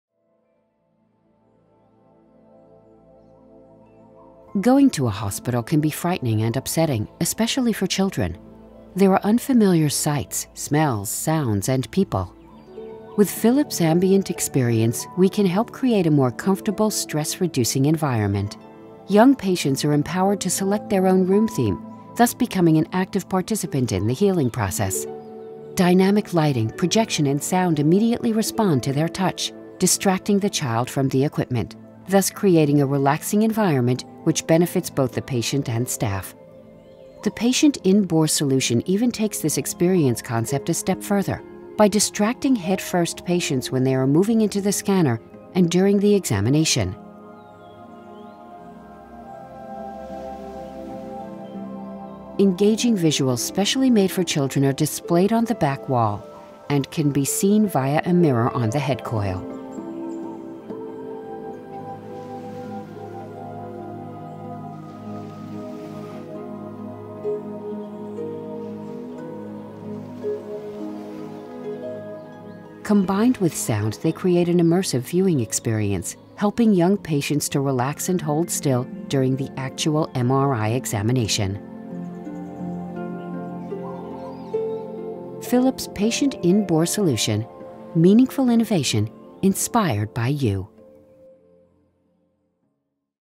Philips Healthcare Consulting and Ambient Experience MRI Patient In-Bore
Philips Ambient Experience can help to create a more comfortable, stress-reducing environment. Young patients are encouraged to select their themes thus becoming an active participant in the healing process. The MRI Patient In-Bore solution takes the  ambient experience concept further by distraction patients when they are moving into the scanner and during the examination, helping young patients to relax and hold still during the MRI examination.